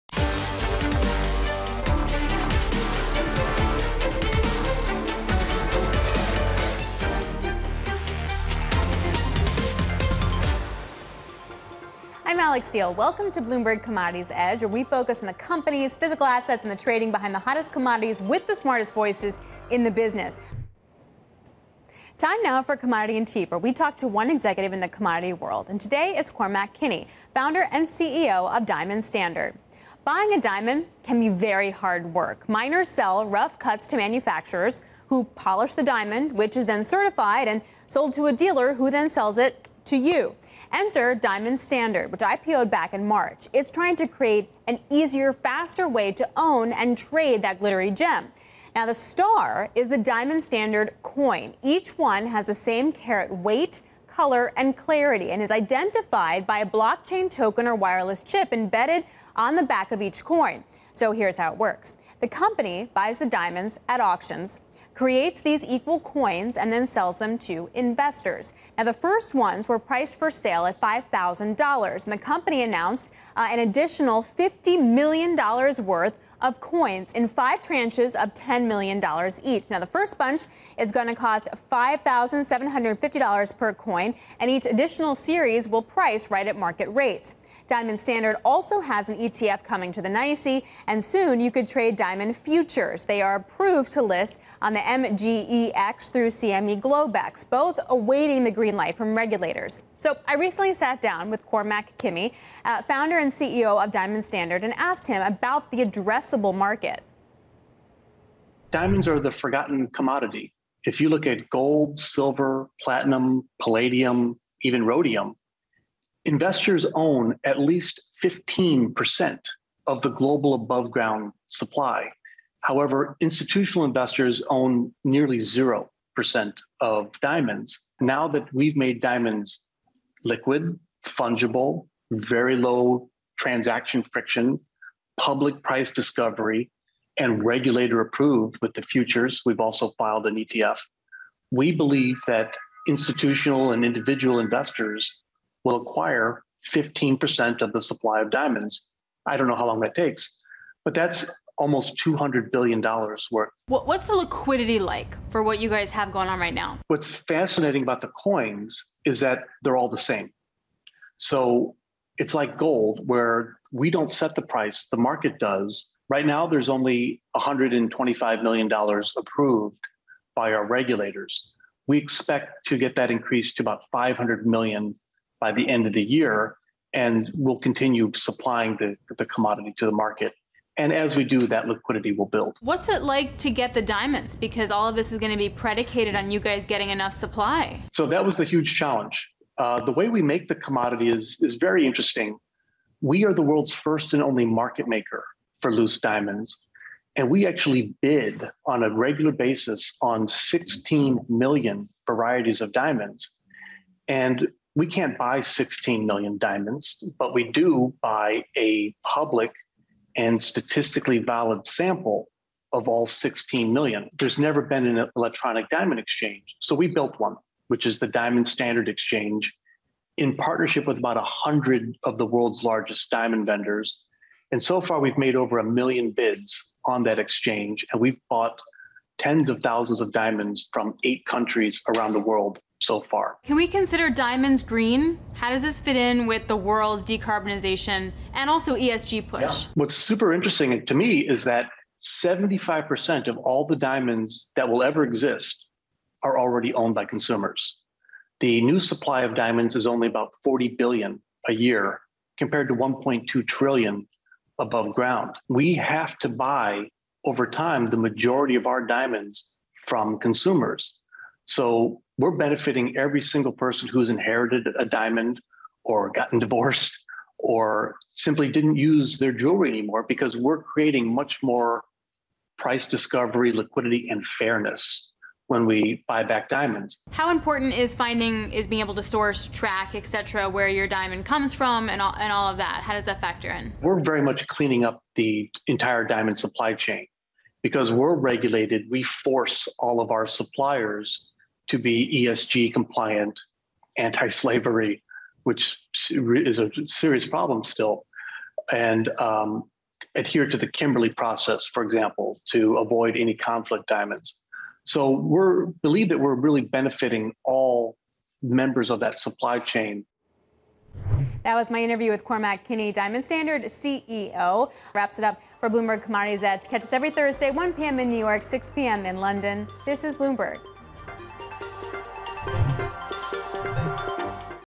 Bloomberg Commodities Edge, Aug. 19 2021 with Cormac Kinney, Founder and CEO of Diamond Standard Co.
Bloomberg's Alix Steel interviews Cormac Kinney, to learn more about Diamond Standard, the first regulator approved diamond commodity. How the commodity provides transparency, price discovery, and liquidity, enabling investors to acquire $200 billion, or 15%, of diamonds. How the diamonds are purchased in a public exchange process. And how Diamond Standard is making diamonds green, acquiring the majority of diamonds from consumers at a fair price.